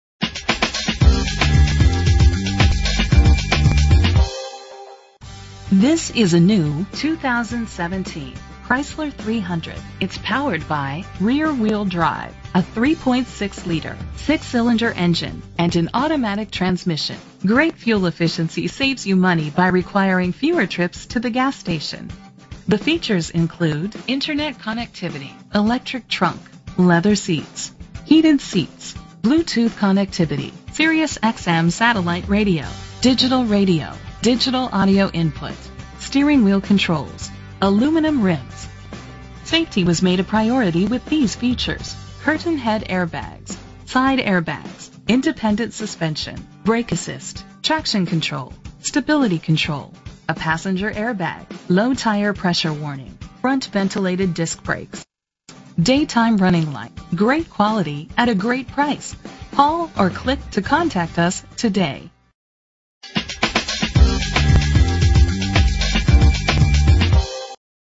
2017 Chrysler 300 - ORLANDO FL
Year: 2017
Make: Chrysler
Model: 300
Trim: Limited
Engine: 3.6 liter 6 cylinder 24 valve
Transmission: 8-Speed Automatic
Color: Velvet Red
Mileage: 3
Address:
9051 EAST COLONIAL DRIVE